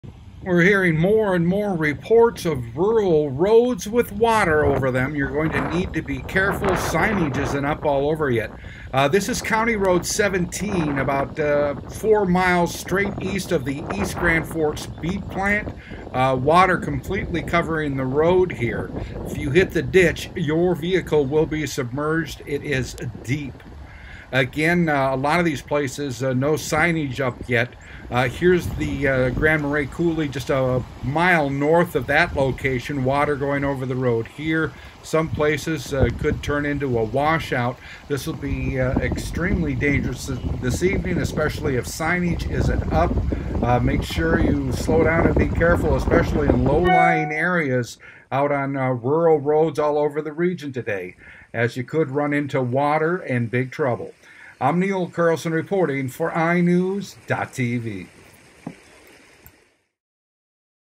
Hazardous Road Conditions Around The Region
EAST GRAAND FORKS, MN (iNewZ.TV) We're hearing numerous reports of water over roads around the region. Be careful driving, especially in rural areas.